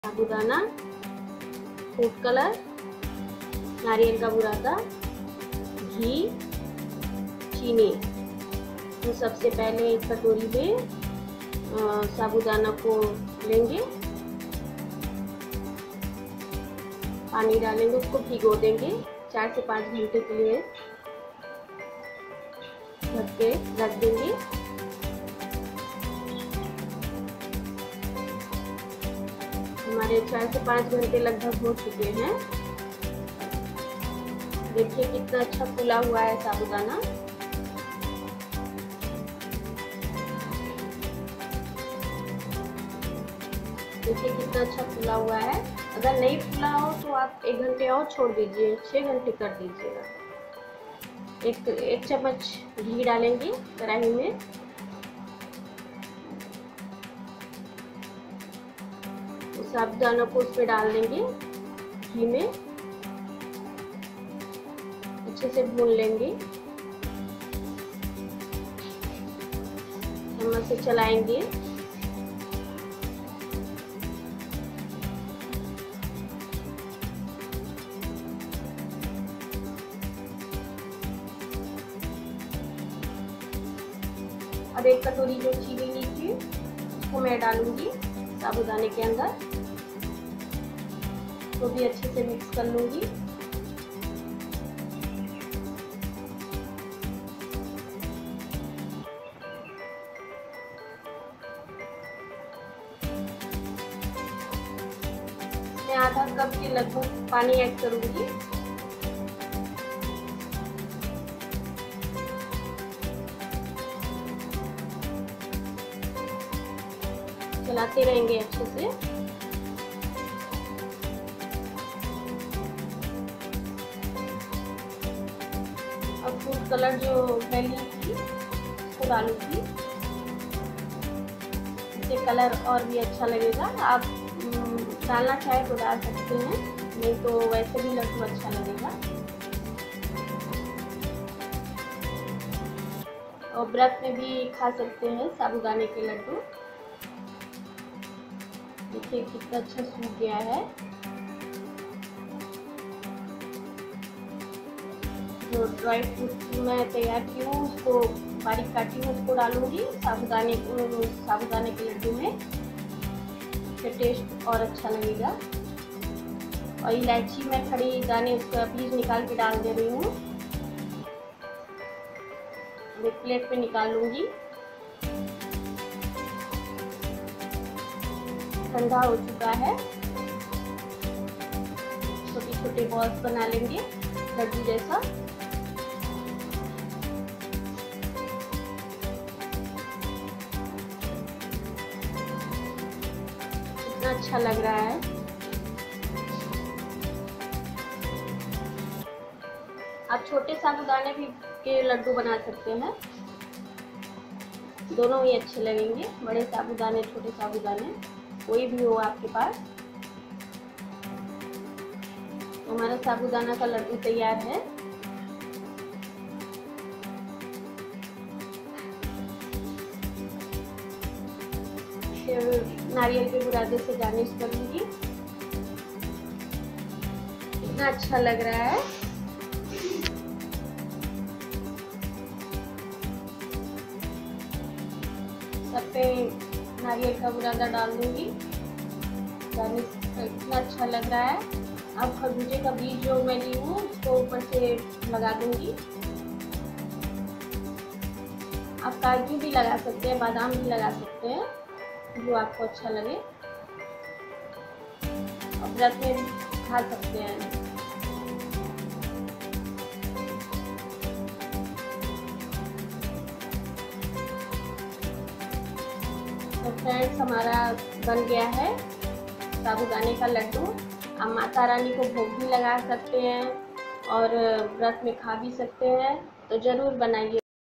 स्वादिष्ट साबूदाने के लड्डू/how to make sabudana laddu /sago laddu
Enjoy this tasty and healthy sabudana(sago) laddu😘😘

INGREDIENTS-
Sabudana(sago) , sugar , grinded coconut , ghee(butter) , food colour